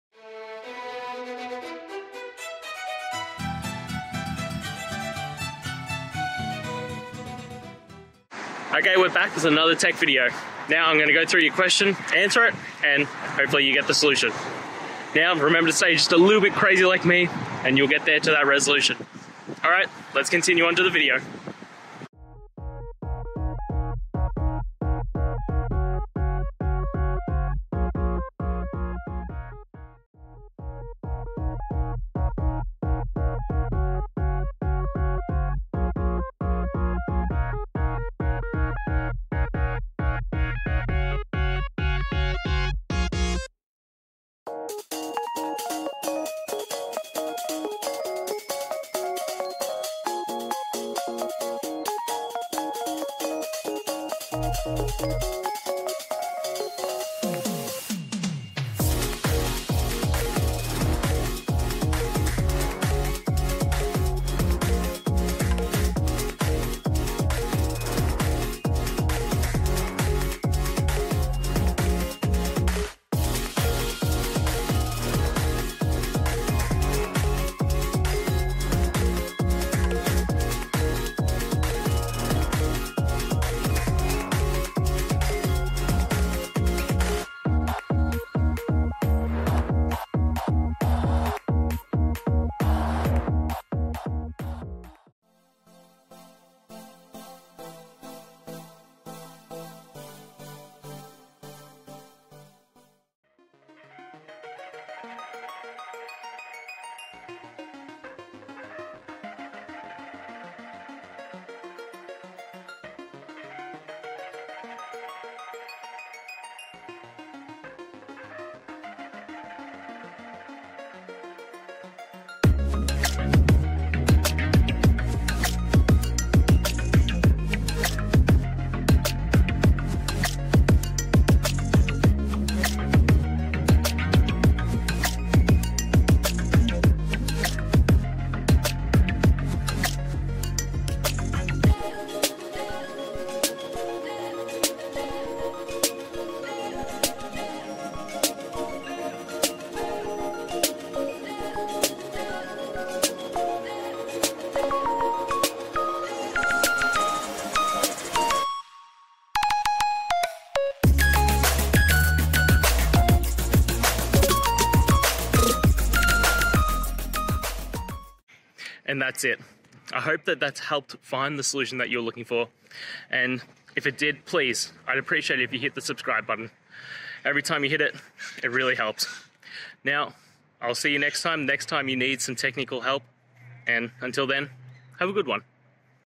How to Omit Struct Field When Marshalling but Keep When Unmarshalling in Go
In this video, we delve into a common challenge faced by Go developers: how to effectively manage struct fields during the marshalling and unmarshalling processes. Specifically, we'll explore techniques to omit certain fields when converting structs to JSON or other formats, while ensuring those fields remain intact when converting back to structs. Join us as we provide practical examples and best practices to streamline your data handling in Go.

Today's Topic: How to Omit Struct Field When Marshalling but Keep When Unmarshalling in Go

Related to